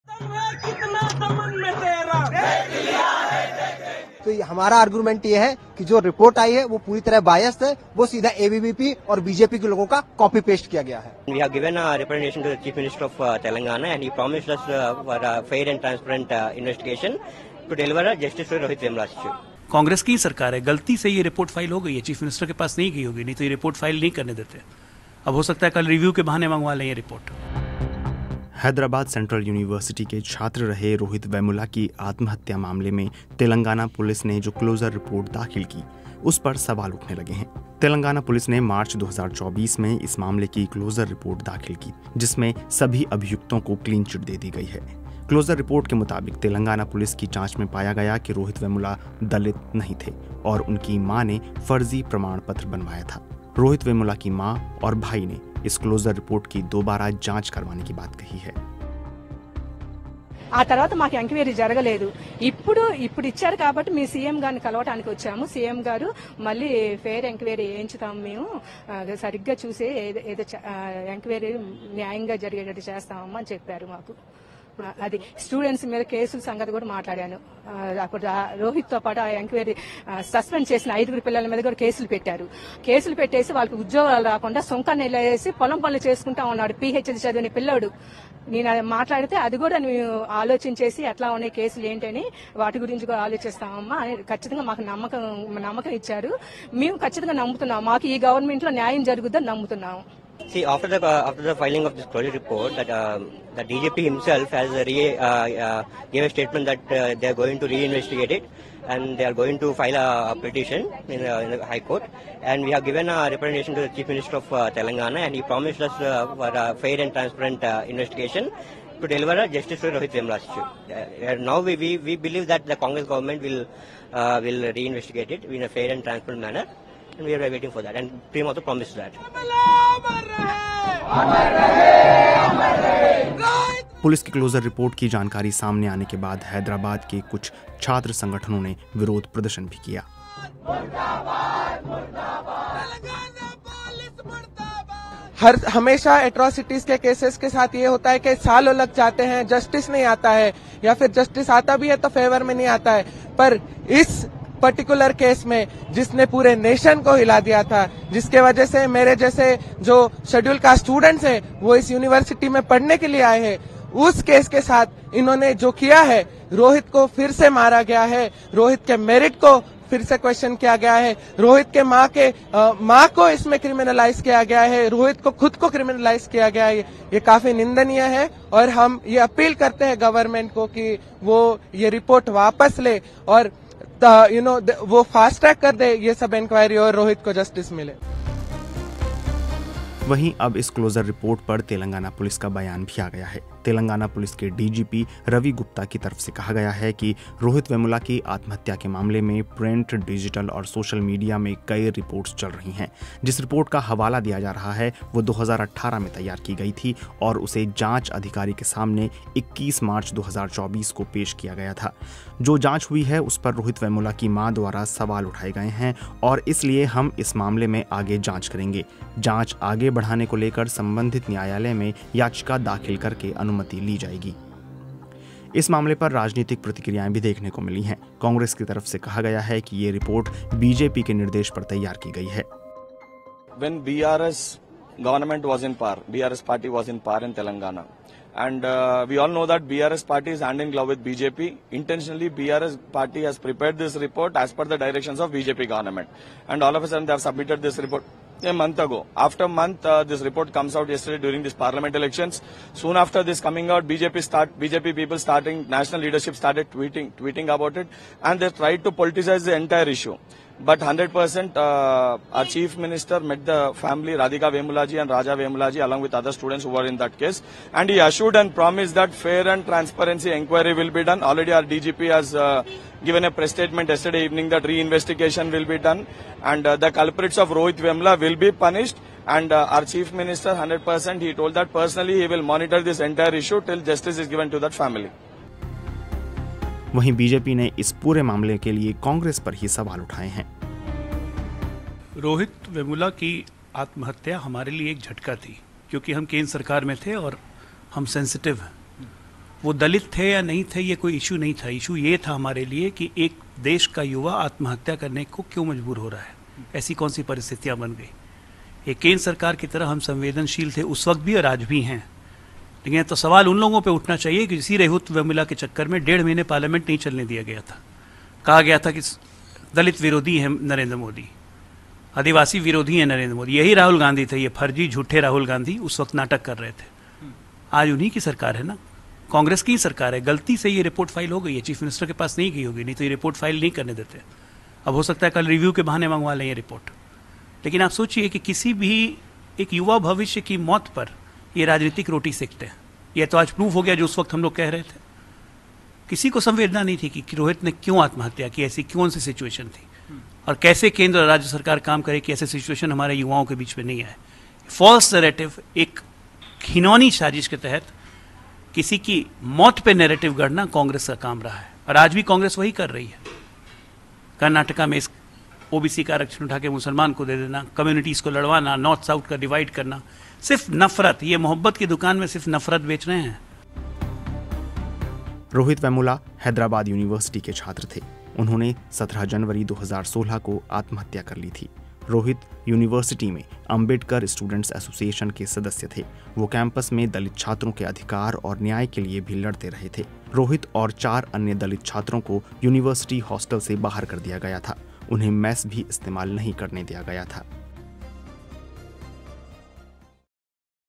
Rohith Vemula मामले की जाँच बंद करने को लेकर उठे सवाल, अब आगे क्या? (BBC Hindi)
हैदराबाद सेंट्रल यूनिवर्सिटी के छात्र रहे रोहित वेमुला की आत्महत्या मामले में तेलंगाना पुलिस ने जो क्लोज़र रिपोर्ट दाख़िल की है, उस पर सवाल उठ रहे हैं. तेलंगाना पुलिस ने मार्च 2024 में इस मामले की क्लोज़र रिपोर्ट दाख़िल की थी जिसमें सभी अभियुक्तों को क्लीन चिट दे दी गई है. क्लोज़र रिपोर्ट के मुताबिक़, तेलंगाना पुलिस की जांच में पाया गया कि रोहित वेमुला दलित नहीं थे और उनकी मां ने फर्ज़ी प्रमाण पत्र बनवाया था. रोहित वेमुला की मां और भाई ने ने इस क्लोजर रिपोर्ट की दोबारा जांच करवाने की बात कही है.

नोटः यदि आपको आत्महत्या के विचार आ रहे हैं या आपकी जानकारी में किसी और के साथ ऐसा होता है, तो आप भारत में आसरा वेबसाइट या वैश्विक स्तर पर बीफ्रेंडर्स वर्ल्डवाइड के ज़रिए सहयोग ले सकते हैं.